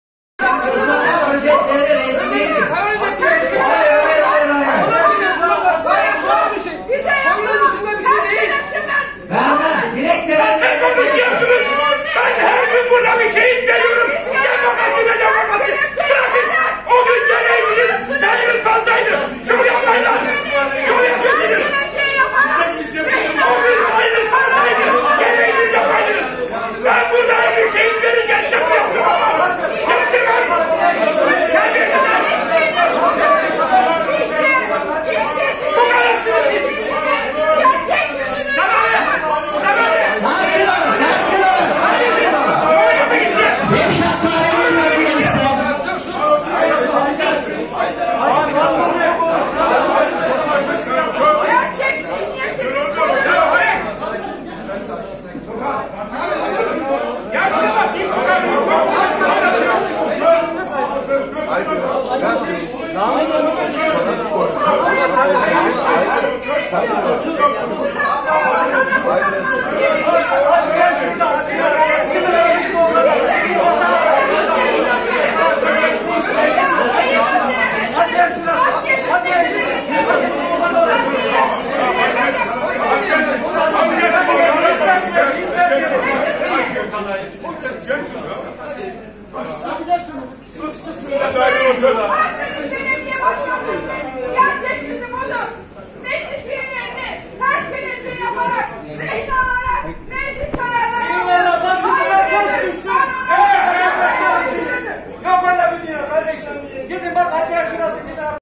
Akdeniz Meclisinde Sert Tartışmalar Gerilime Neden Oldu.
Akdeniz Belediye meclisinde geçen ay HDP’li 5 meclis üyesinin gözaltına alınması sonrası gerilim sürüyor. Bugün yapılan toplantıda HDP- CHP cephesinin geçen ay ki kararların özetine şerh düşmek istemesi üzerine gerilim yaşandı.